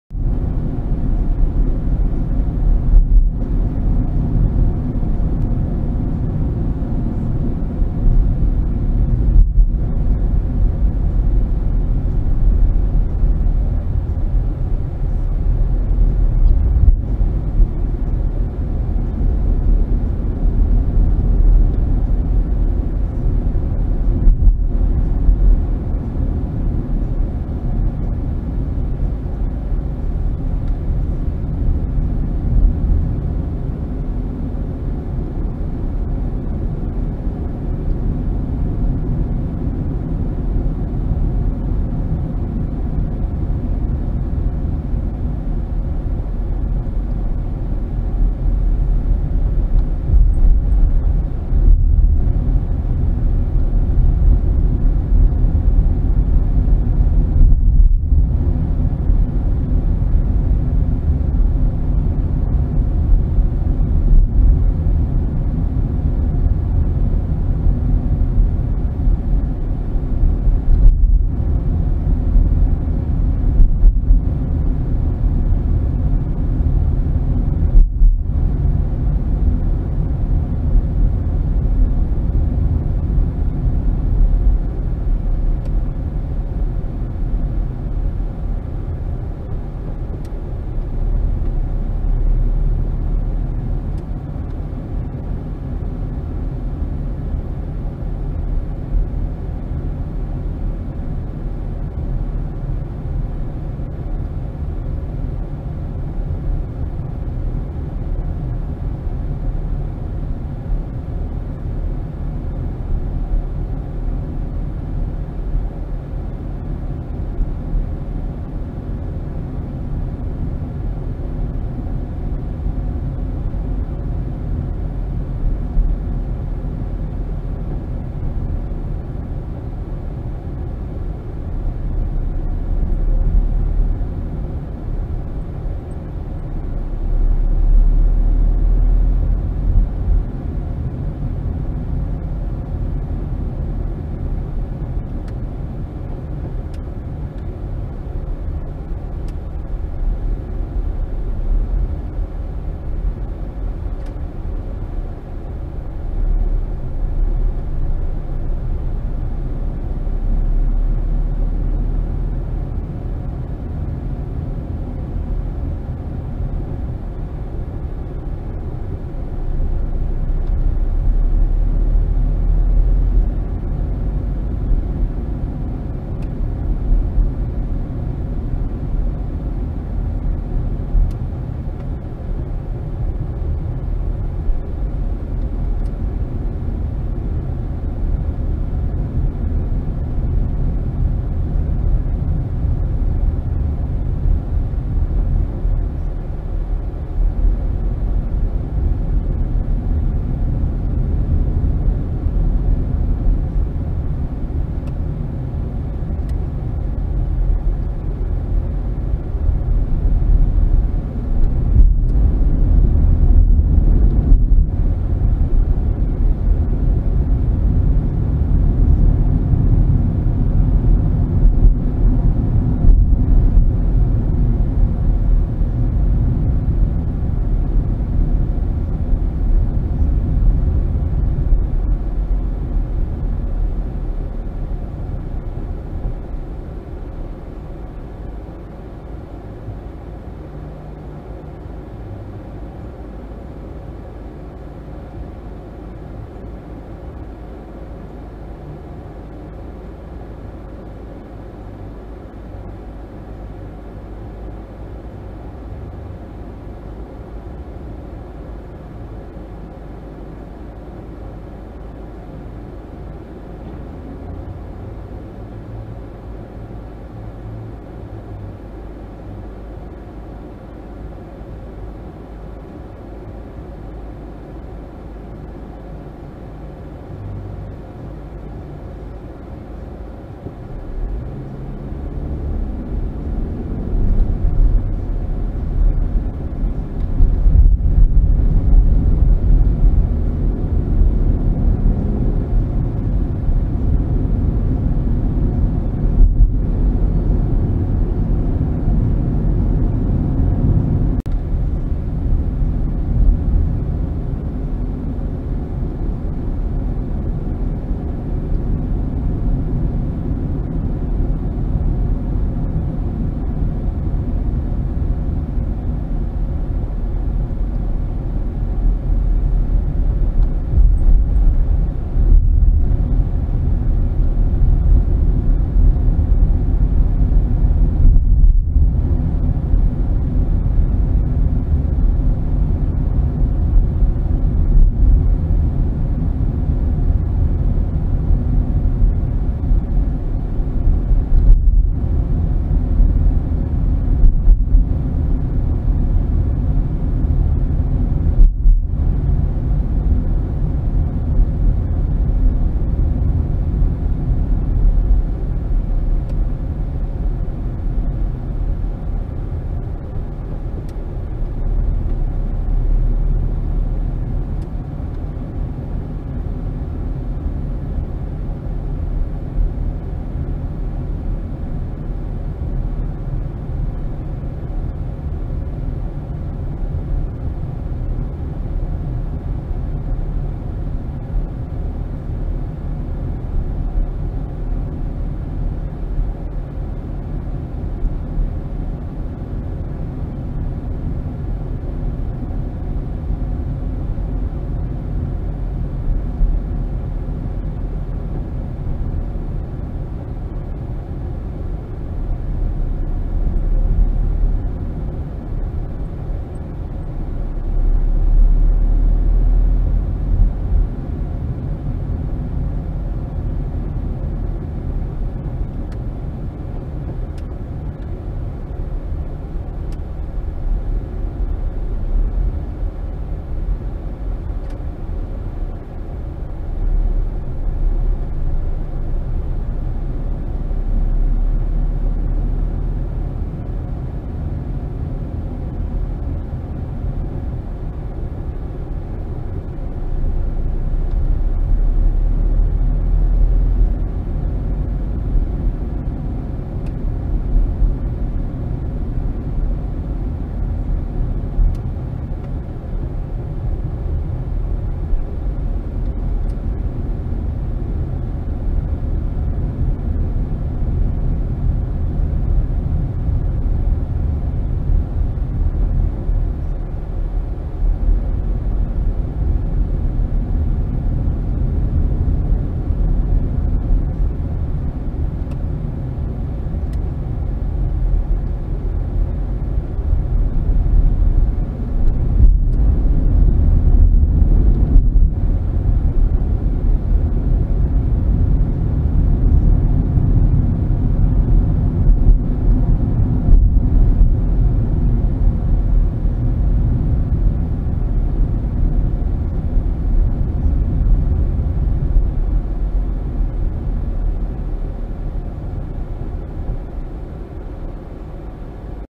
Antrim Town 2020
For all the great people that emigrated from the town many years ago this is the town today 2020